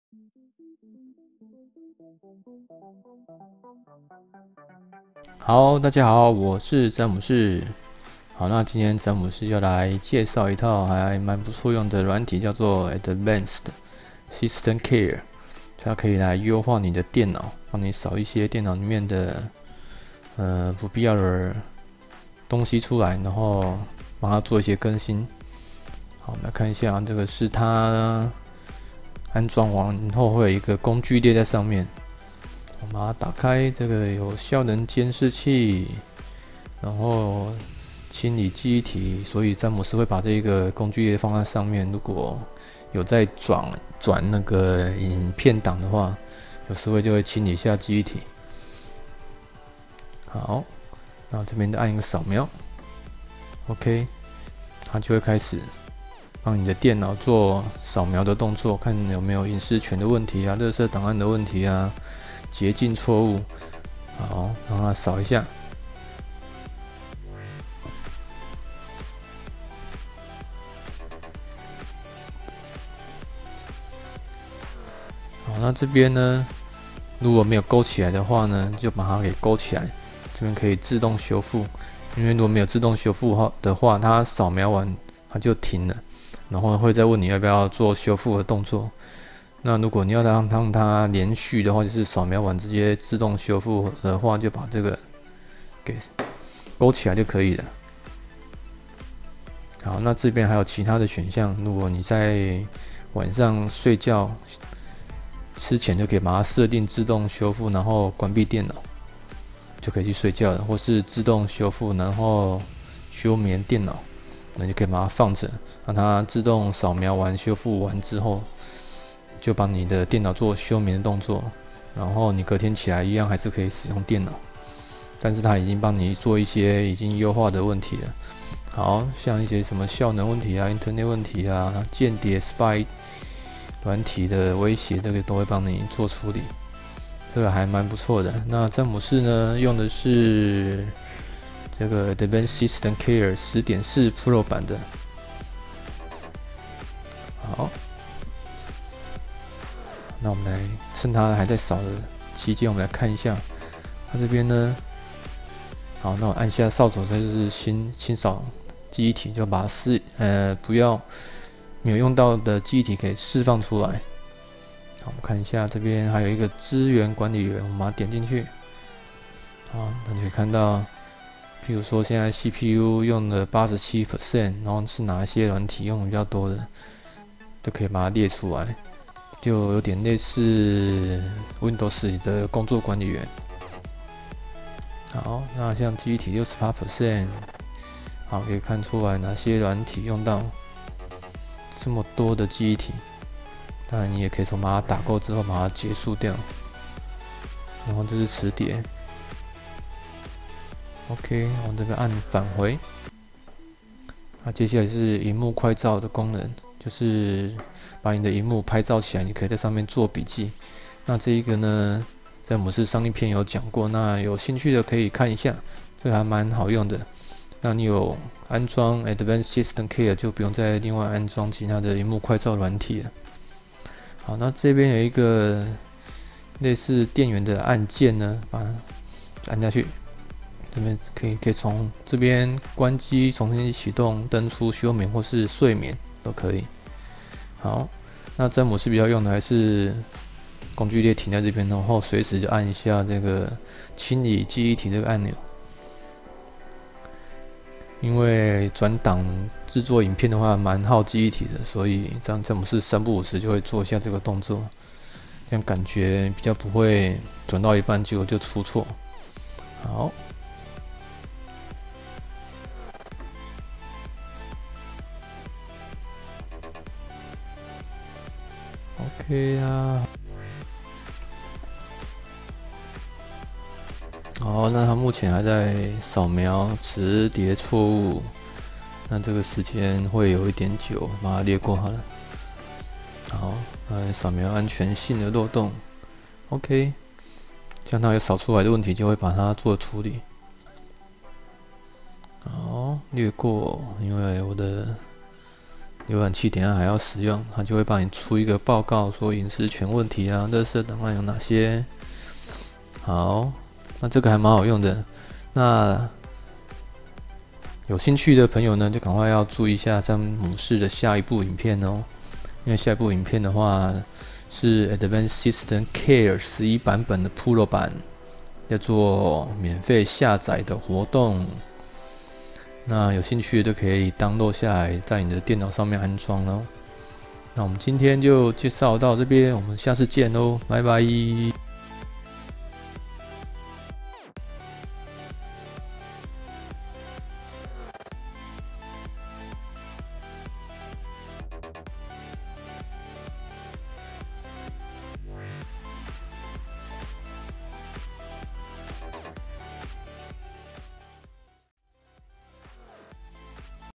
好用的電腦優化軟體!! Advanced SystemCare 10.4 Pro版介紹 [好用的電腦軟體] [1080P HD] [宅爸詹姆士]
好用的電腦優化軟體!! Advanced SystemCare 10.4 Pro版本介紹,你也來優化你的電腦吧, 試試看吧, GO

[Review] Advanced SystemCare.10.4 Pro  [1080P HD]